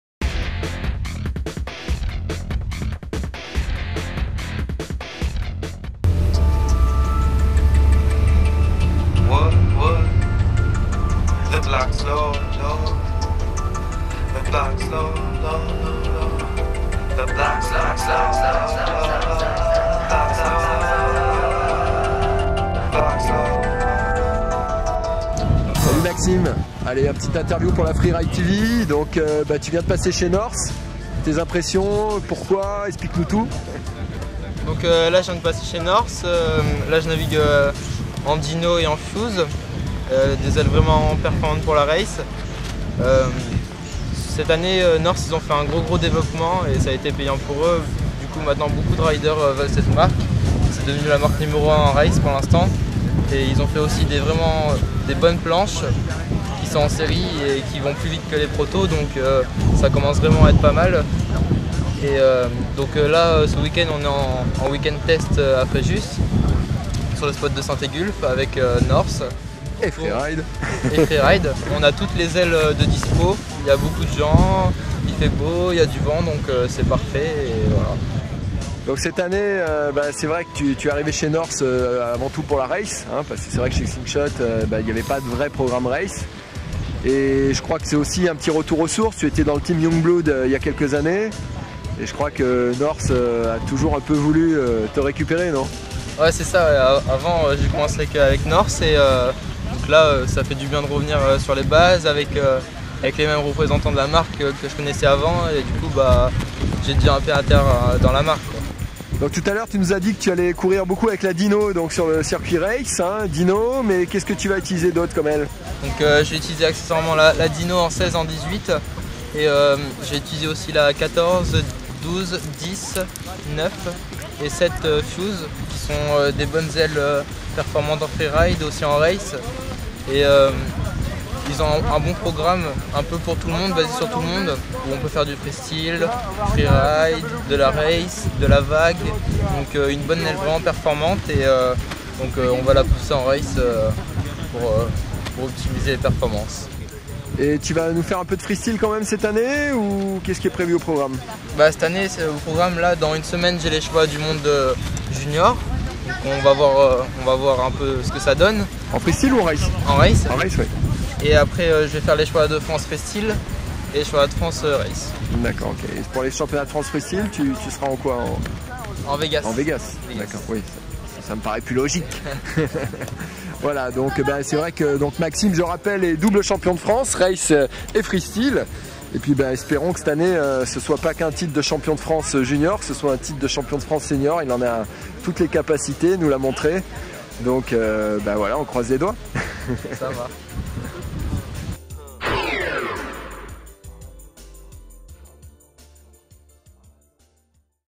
Interview Maxime Nocher - Par Freeride Attitude
Notre équipe à rencontré Maxime Nocher durant la journée de tests North à Saint Aygulf le 7 et 8 mai 2011.

Caméra : Cyril Garbous, Jéremy Barralis.
Montage : Bernaert Baptiste